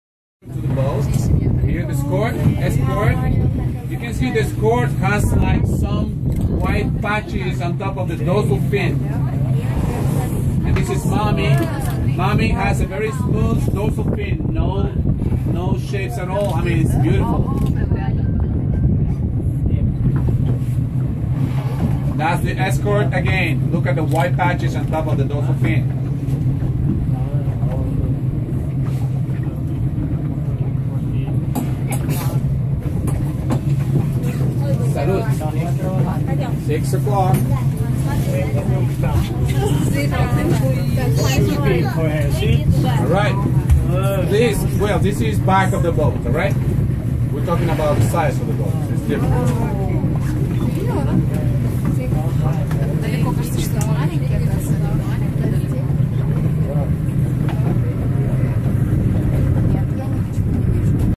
Whale Watching in the Dominican Republic
Whale Watching in the Dominican Republic at Samana location.Closer look.Mother ,baby and escort